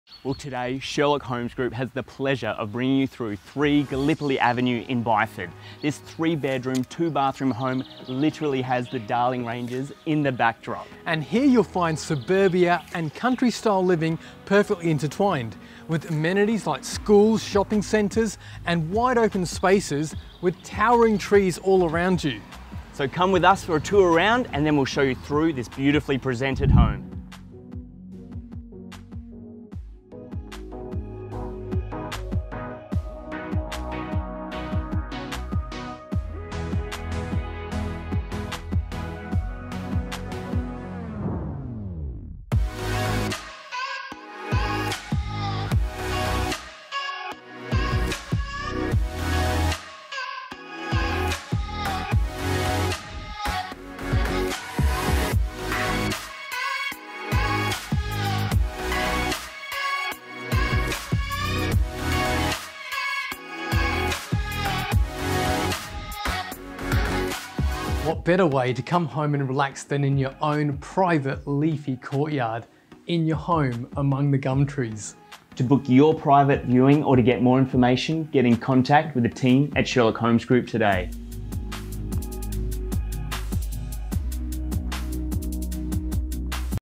3 Gallipoli Ave, Byford Video Tour
Byford is a rapidly growing community that has embraced change and held onto that country township feeling. When talking to locals around the area, who are happy to stop and have a chat, there is an overwhelming feeling of pride and strong family ties that ripples through this picturesque suburb.

Too many suburban developments are popping up that seem identical to the last one and lack any real point of difference. Here the Byford locals all have a resounding message, that this area has a “suburban country vibe”.

This home is the epitome of this, overlooking a green open space in the midst of towering trees that share their shadow with the local shopping and community hub directly across the footpaths. Convenient amenities are a stones throw away!
Situated on a 325m2 block, the moment you enter swinging open the huge pivot vaulted entry door, this custom quality built Summit Home has been designed to offer practical low maintenance living in a resort style environment. The grand entrance foyer is a canvas that is adorned by art and images that portray style and class.

Walking through Ice-cold air conditioning humming and the whirl of a ceiling fan fringed with greenery instigate an instant sigh of relief when you are welcomed by the open plan living area that wraps around the magical alfresco courtyard.
The finish and detailed touches are oozing quality and this theme runs from one room to the next with a beautifully designed floor plan that is far from a run of the mill 3x2 home.

First class features like premium flooring and lighting with elegant cabinetry and with too many features to list the care and thought to accents and design are everywhere. This is your opportunity to make this one yours knowing all the hard work has been done for you to enjoy.

Local School Catchments:

Beenyup Primary School - 0.94 km

West Byford Primary School - 1.16 km

Byford Secondary College - 1.55 km